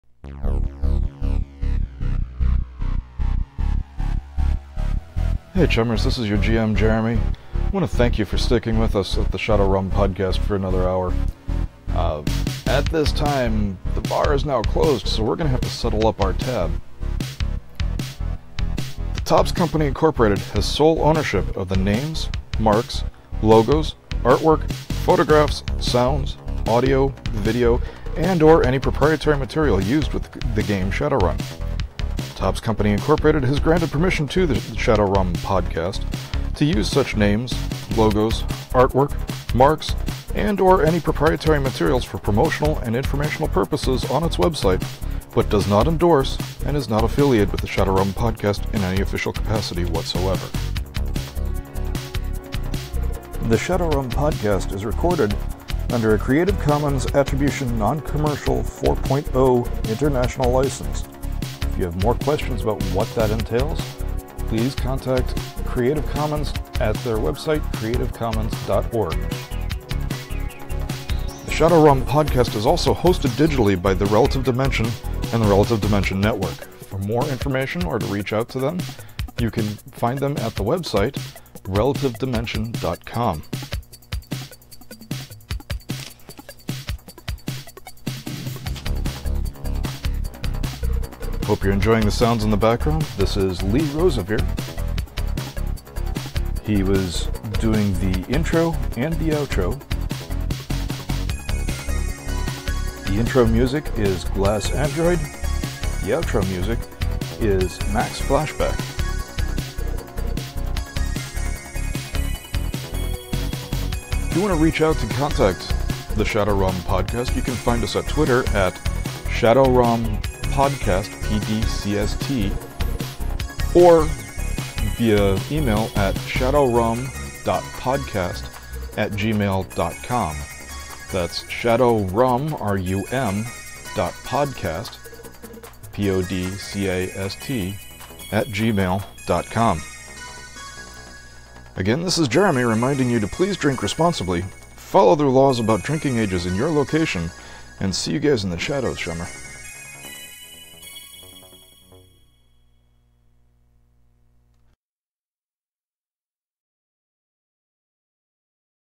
Shadowrum, Episode 008. It's Gonna Be One of THOSE Nights.
Episode 008: It's gonna be one of THOSE nights.  The two game groups merge for one night, and we have our first Guest GM: Lawrence! The team is going to be in Atlanta (I apologize in advance), and some familiar voices will be playing different characters.  Stick around as these folks try to find out if they can handle working together.  Oh, and targeting a mark for an extraction.